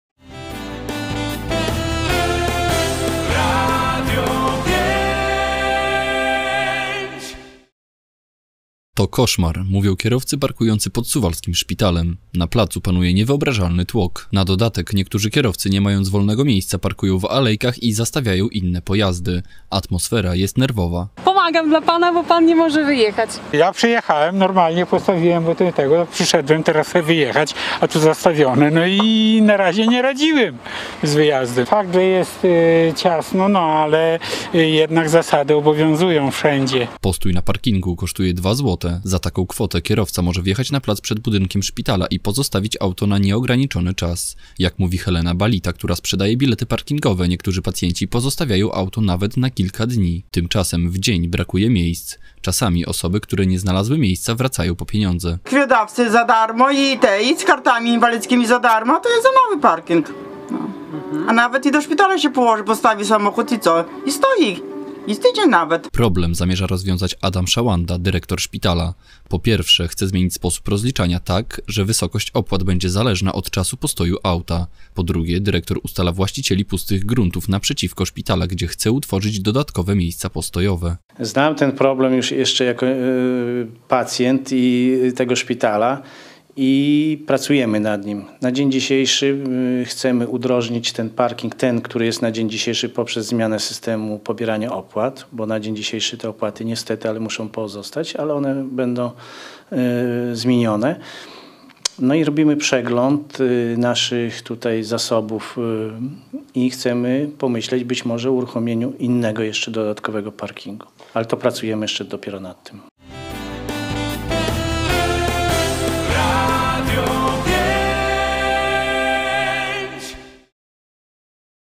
Tłok na parkingu przed szpitalem
- To koszmar - mówią kierowcy parkujący pod suwalskim szpitalem. Na placu panuje niewyobrażalny tłok. Na dodatek niektórzy kierowcy nie mając wolnego miejsca parkują w alejkach i zastawiają inne pojazdy. Atmosfera jest nerwowa. Postój na parkingu kosztuje 2 złote. Za taką kwotę kierowca może wjechać na plac przed budynkiem szpitala i pozostawić auto na nieograniczony czas. Jak mówi Helena Balita, która sprzedaje bilety parkingowe, niektórzy pacjenci pozostawiają auto nawet na kilka dni. Tymczasem w dzień brakuje miejsc. Czasami osoby, które nie znalazły miejsca, wracają po pieniądze. Problem zamierza rozwiązać Adam Szałanda, dyrektor szpitala. Po pierwsze chce zmienić sposób rozliczenia tak, że wysokość opłat będzie zależna od czasu postoju auta. Po drugie dyrektor ustala właścicieli pustych gruntów naprzeciwko szpitala, gdzie chce utworzyć dodatkowe miejsca postojowe.